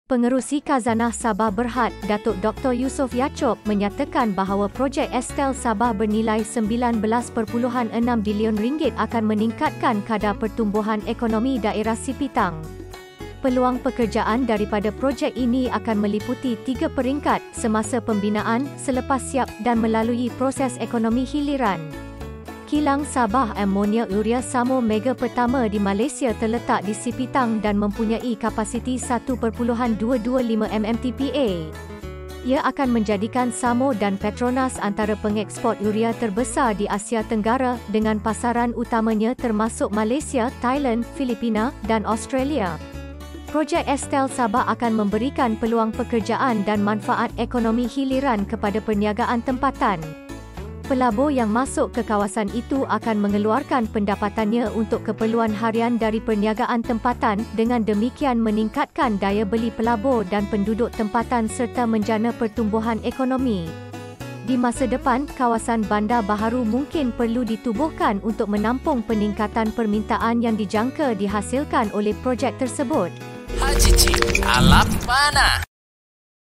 20,000 peluang pekerja baharu di SOGIP Sipitang
SOGIP akan menawarkan 20,000 peluang pekerjaan dan membabitkan nilai pelaburan sebanyak RM19.6 bilion di Taman Perindustrian Minyak dan Gas Sipitang. Ini membawa kebanggaan kepada penduduk setempat kerana akan melonjakkan kadar pertumbuhan ekonomi daerah Sipitang dan memberikan proses pertambahan hiliran ekonomi.